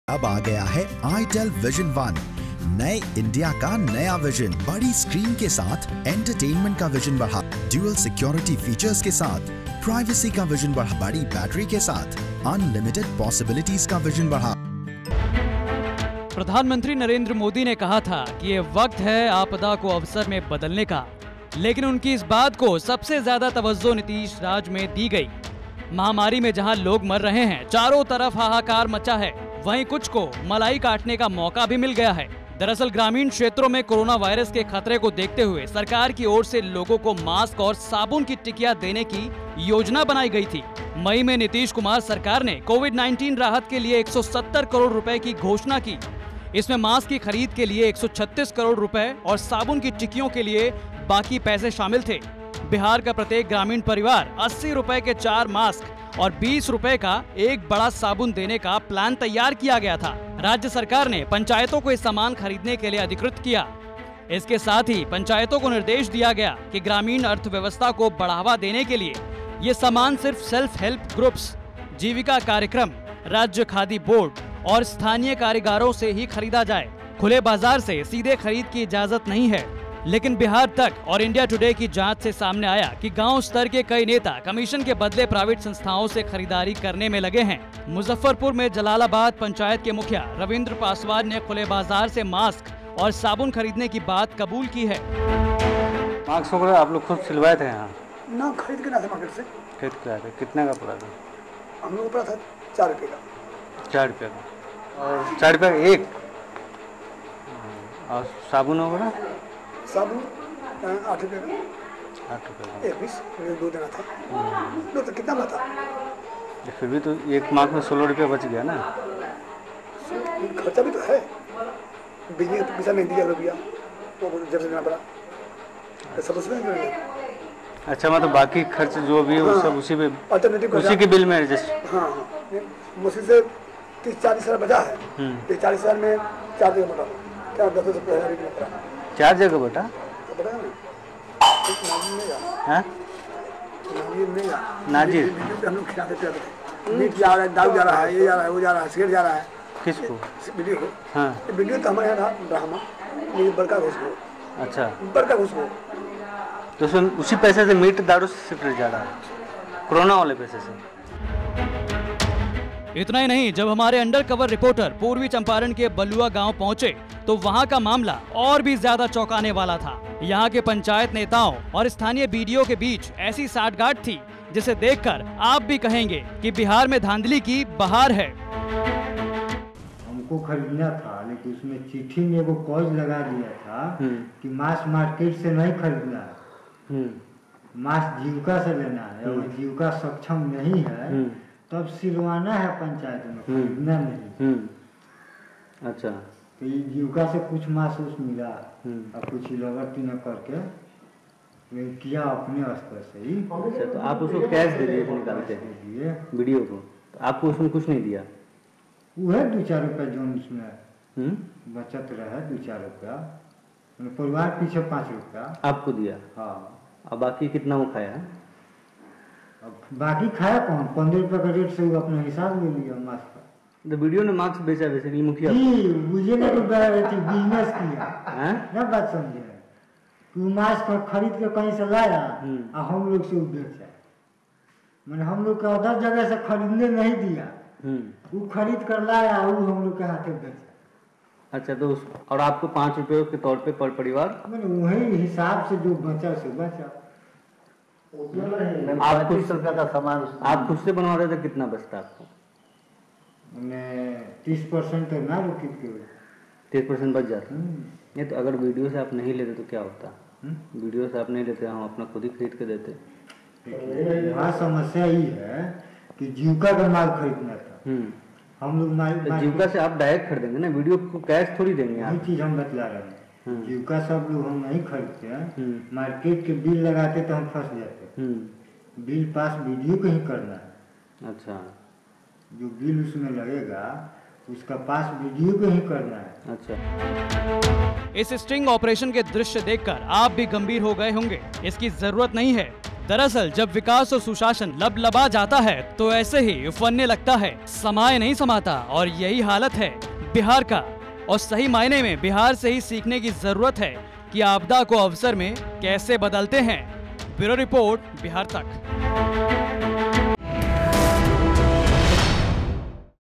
Corona scam सरकारी आधिकारी और पंचायत के सदस्यों के गठजोड़ ने राहत सामग्री में लगाया चूना
बिहार को महामारी और बाढ़ का सामना ही कम नहीं था कि इसके कुछ हिस्सों में कोरोना राहत के लुटेरों ने अपना जाल बिछा दिया । बिहार तक की जांच में खुलासा हुआ है कि किस तरह ग्रामीणों को मुफ्त दी जाने वाली राहत सामग्री में कुछ शातिरों ने सेंध लगाकर अपनी जेबें भरीं ।